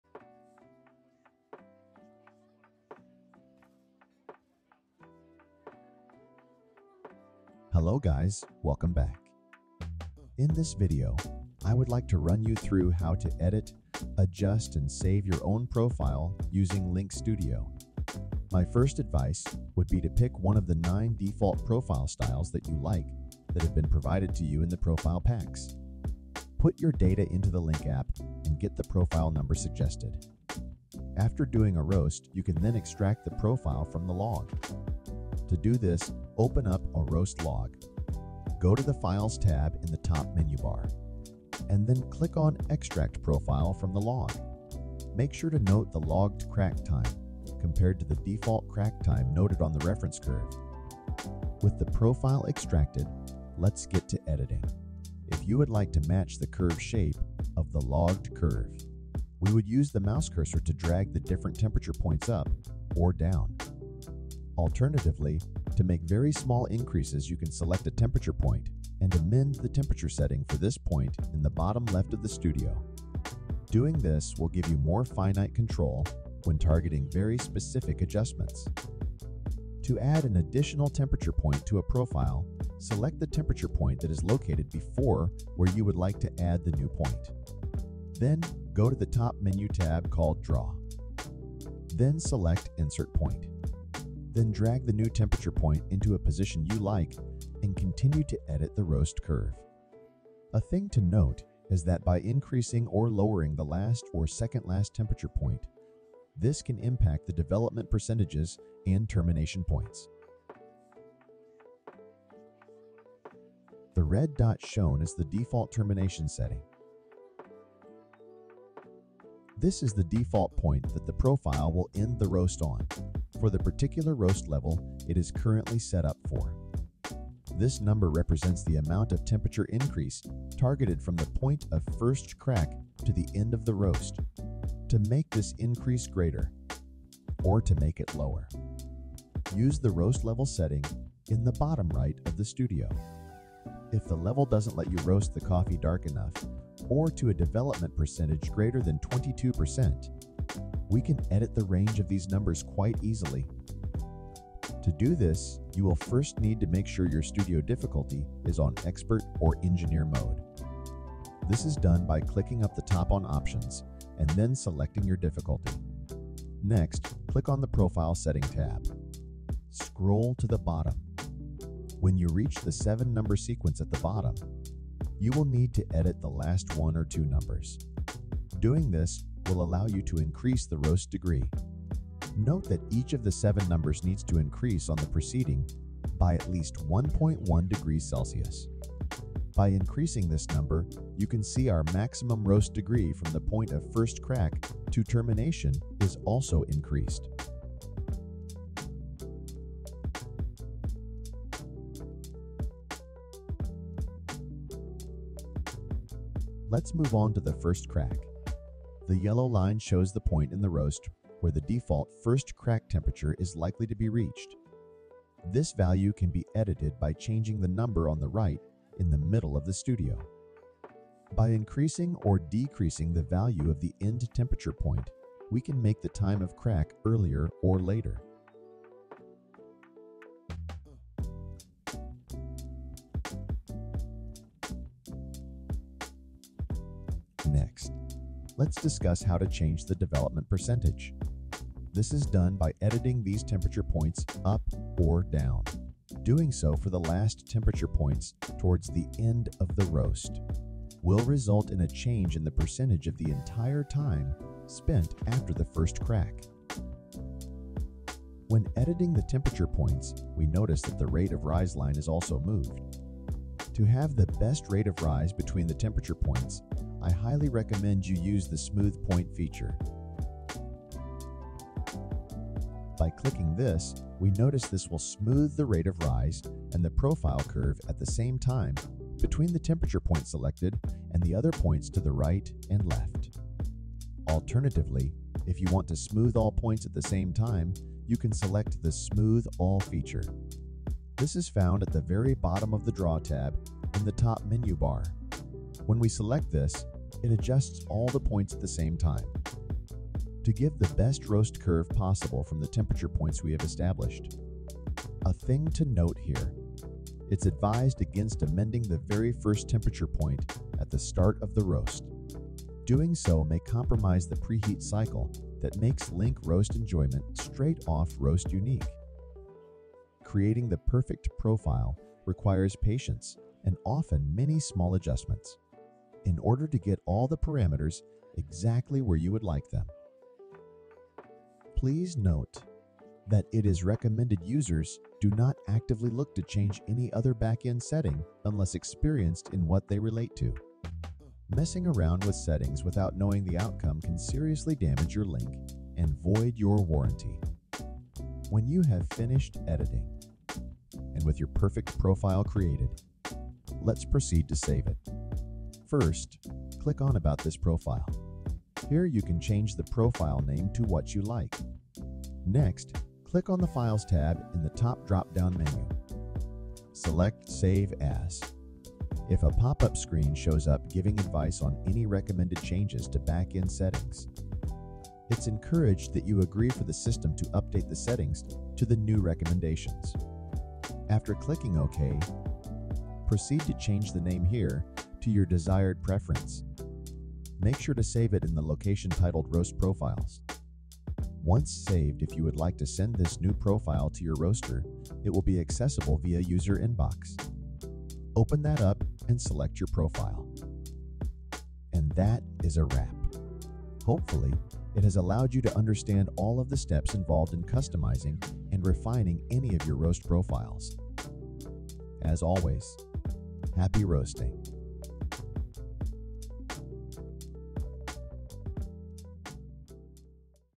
LINK sample roaster – Using LINK studio to make or edit a profile
00:00 intro
00:18 Exporting a profile from a log.
00:53 moving the temperature points.
01:36 Roast level and termination point
02:23 How to let a profile roast darker.
03:28 Editing first crack
03:57 Development percentages
04:18 Using smooth point feature.
05:04 Advice on starting temperature point of the profile
05:50 Saving the profile to your user inbox.
06:41 Outro